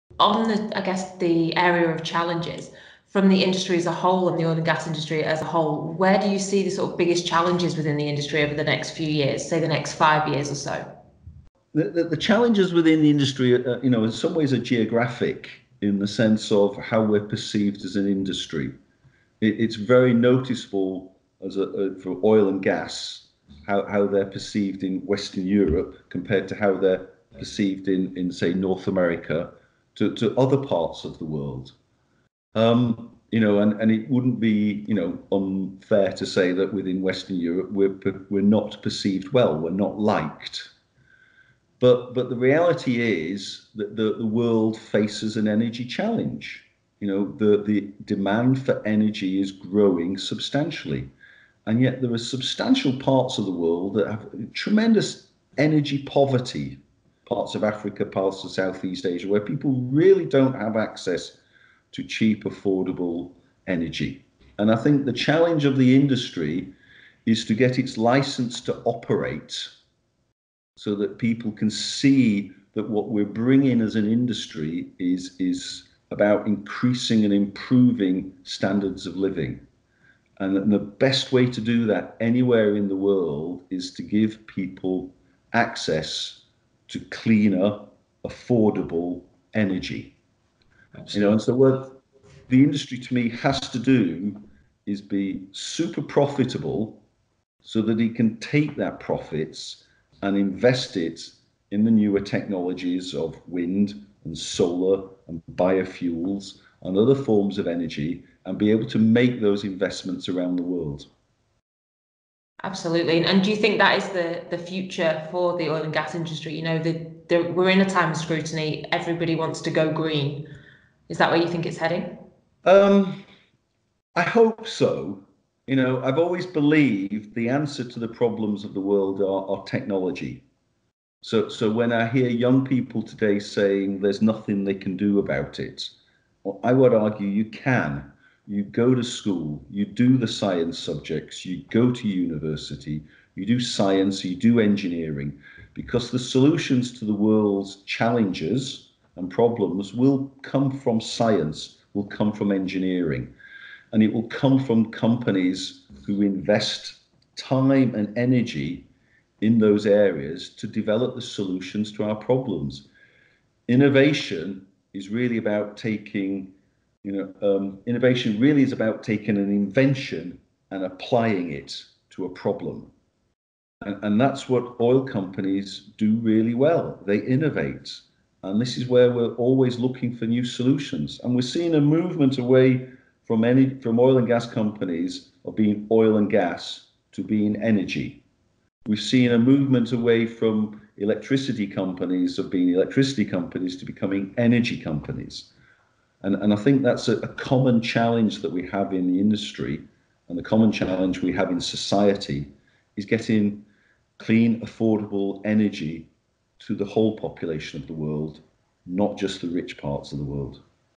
Oil & Gas Industry Challenges 2020 | Nick Maden | 6 Group
What are the Oil & Gas challenges for 2020 and beyond?

In the second part of our podcast with Nick Maden, former SVP International Exploration for Equinor, he gives his thoughts on the current and future challenges facing the Oil & Gas industry.